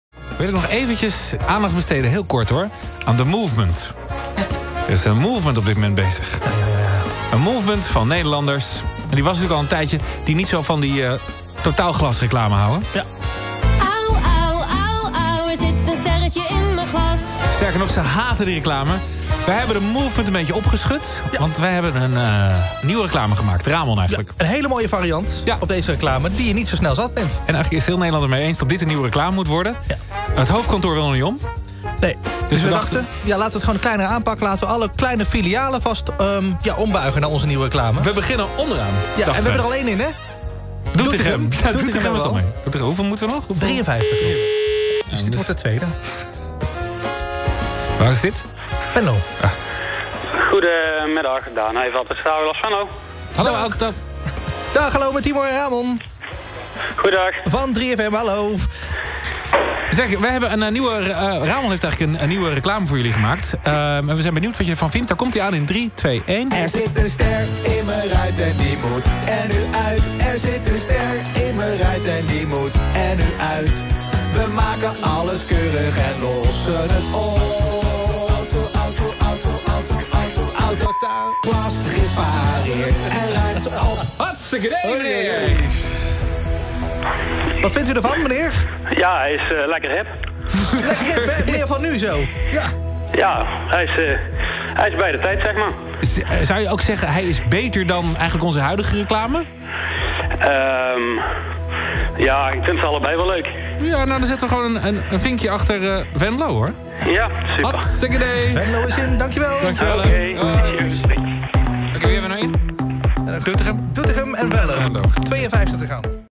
Autotaalglas soap: Ook Venlo kunnen wij afvinken!
Het filiaal van Autotaalglas Venlo vindt de nieuwe reclame 'Lekker Hip!'